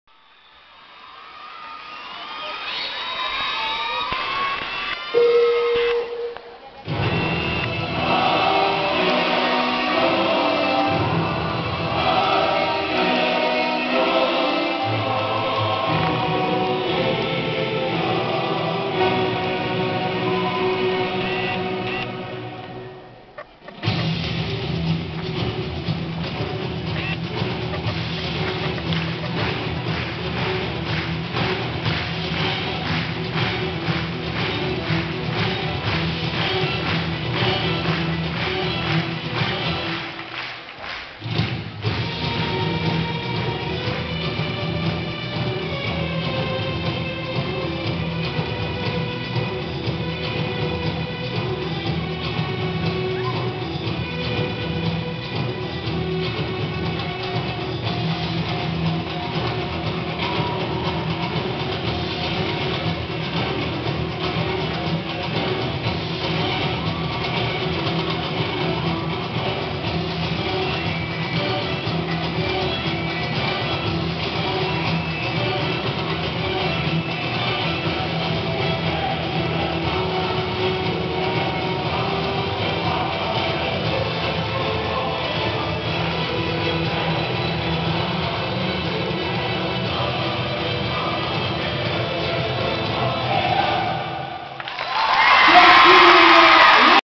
delphine ledoux
premier passage en démonstration de la chamionne de france delphine ledoux, pour les championnats de france organisés par BGR au palais des sports.